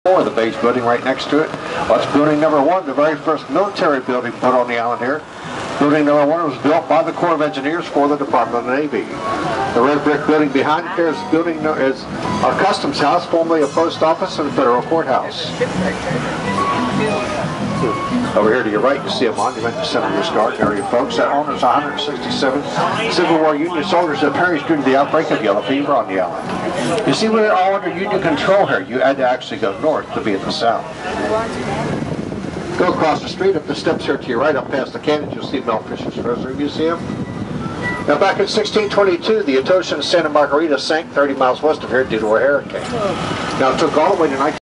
Key West Conch Train Trolley Tour
This was right after we boarded the famous Key West Conch Tour "Train", which was a tram that took guests around the island.  The entire tour lasted about 1 1/2 hours, and you could get off at one of two stops.  See some pictures here: